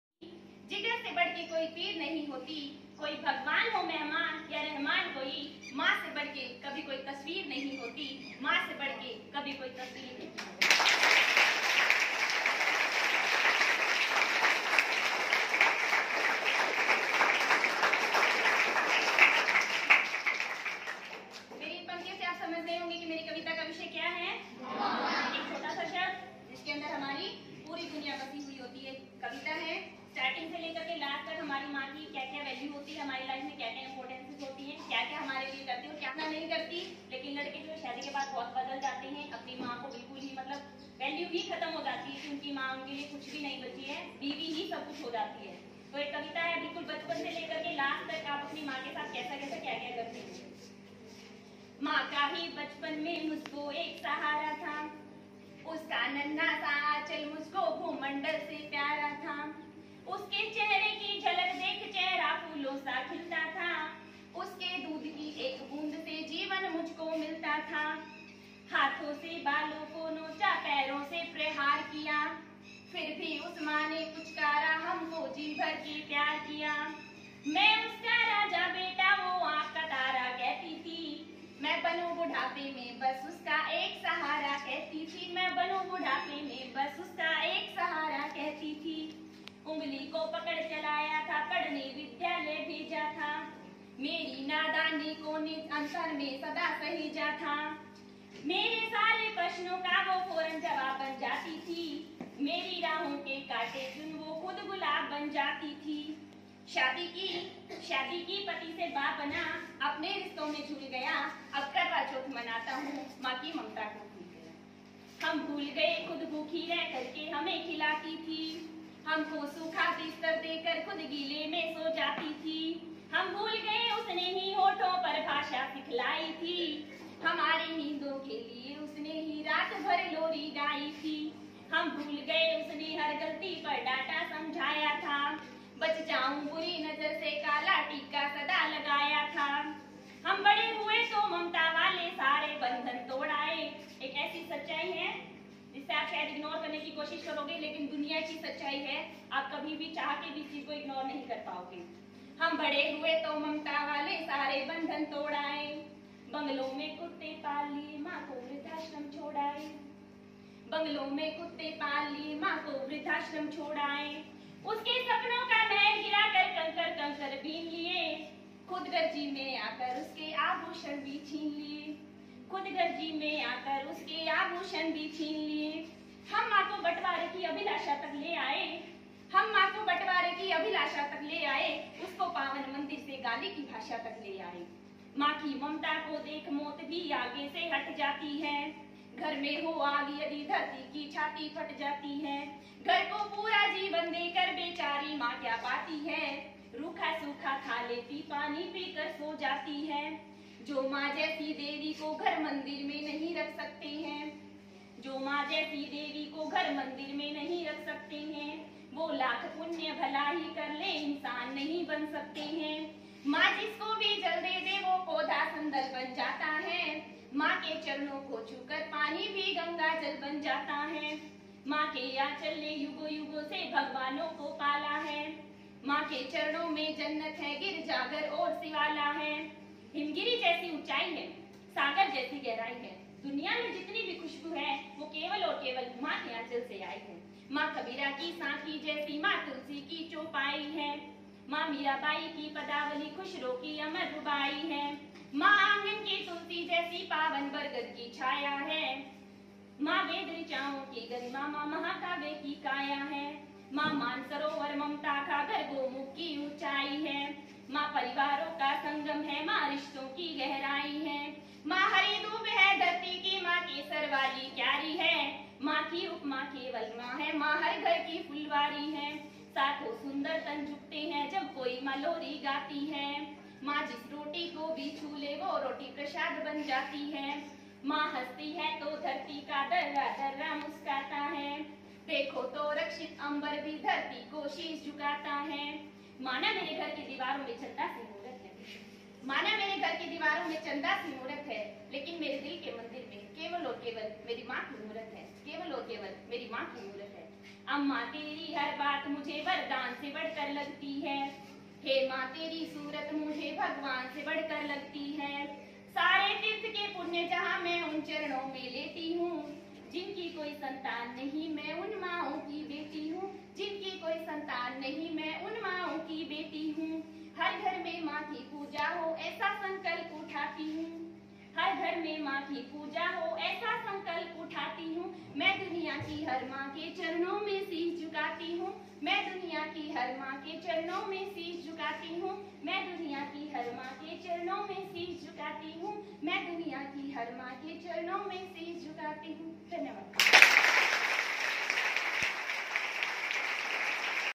सबका दिल जीतने वाली कविता
Hey friends welcome to our new vlog.
डॉ सुनील जोगी जी द्वारा लिखित माँ के ऊपर एक ऐसी कविता जो आपका दिल छू लेगी।उनके शब्दों को गाने की ,सुनाने की एक छोटी सी कोशिश की है।सिर्फ ये सब बोल पाने से ही मैं कोई कवियत्री नहीं बन सकती गुरु जी ।कवियत्री तो क्या मैं तो आपकी चरणों की धूल के समान भी नहीं हूं।🙏🙏🙏🙏🙏🙏

दासता से बड़ी कोई जंजीर नहीं होती।
जिगर से बढ़ के कोई पीर नहीं होती।
कोई भगवान हो मेहमान या रहमान कोई
माँ से बढ़ के कभी कोई तस्वीर नहीं होती।।।।।।।

सारे तीर्थ के पुण्य जहाँ मैं उन चरणों में लेटी हूँ।

जिनकी कोई संतान नहीं मैं उन माओ की बेटी हूँ।

हर घर में माँ की पूजा हो ऐसा संकल्प उठाती हूँ।

मैं दुनिया की हर माँ के चरणों में शीश झुकाती हूँ।।।।।।।।।।

Follow your favourite couple on Instagram.
@ankur.narwal 549.